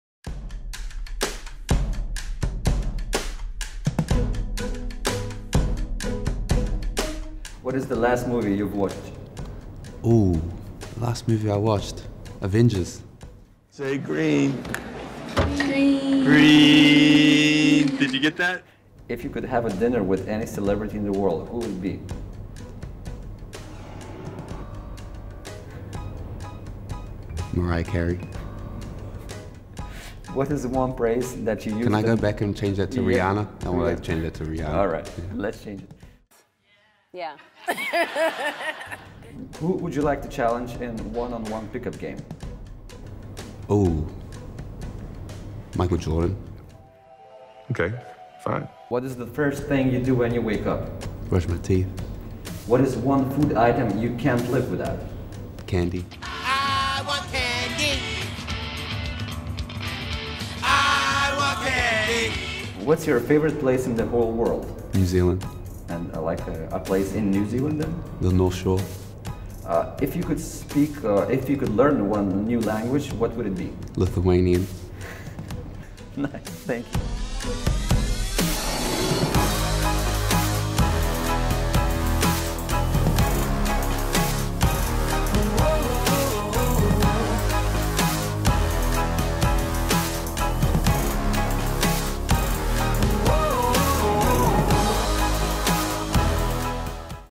60SecondsWithZalgiris Se04 Ep04 (Tai Webster)
Zalgiris' guard Tai Webster answered a number of questions in an episode of Zalgiris TV's 60SecondsWithZalgiris.